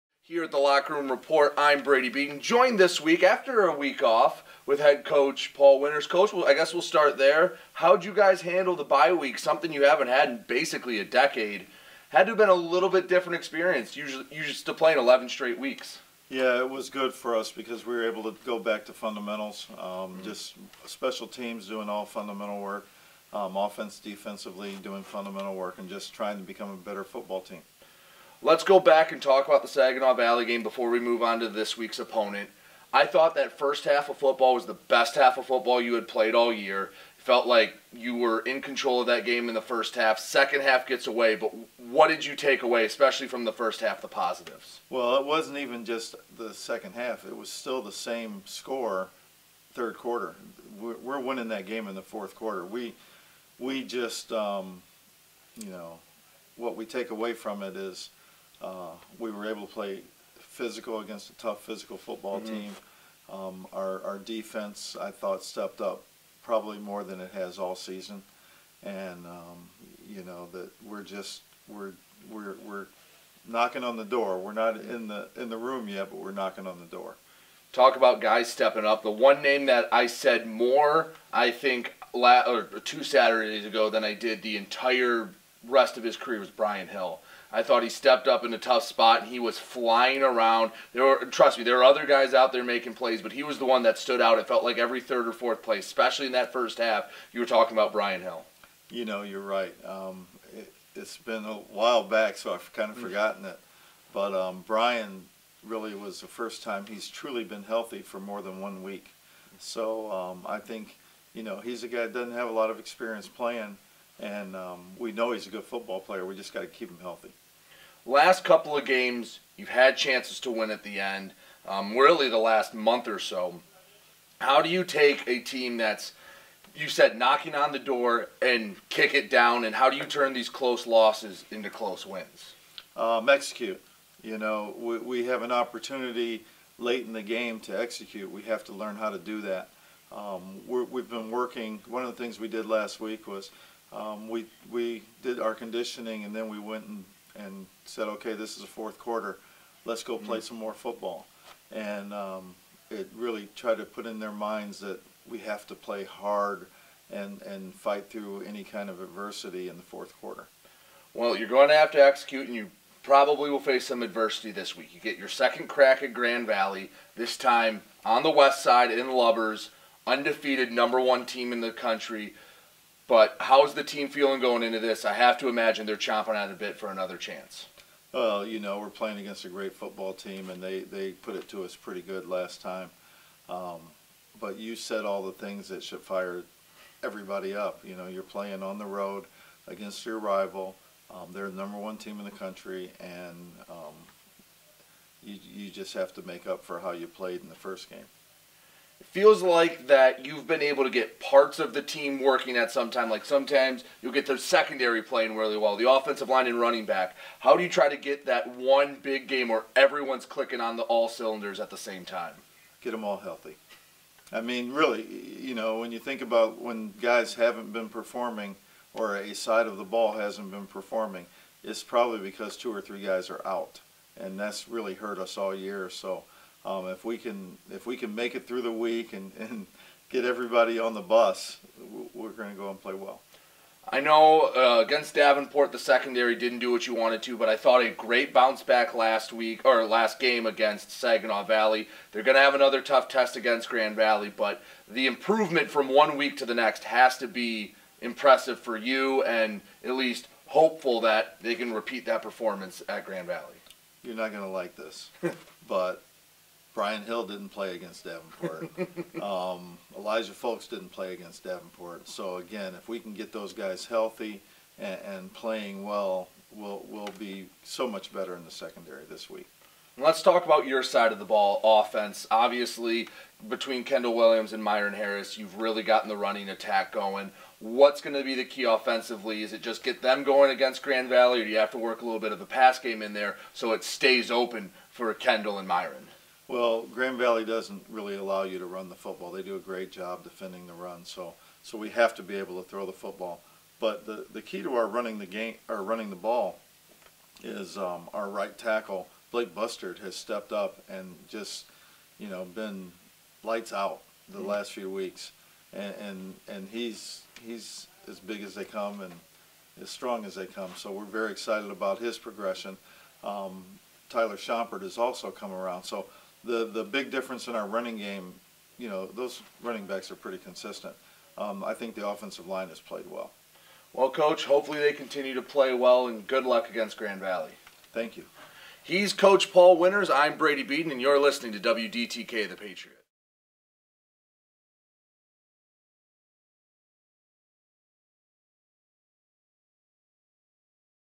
Locker Room Report - Paul Winters (Nov. 1, 2022)
Paul Winters discusses the upcoming game against Grand Valley State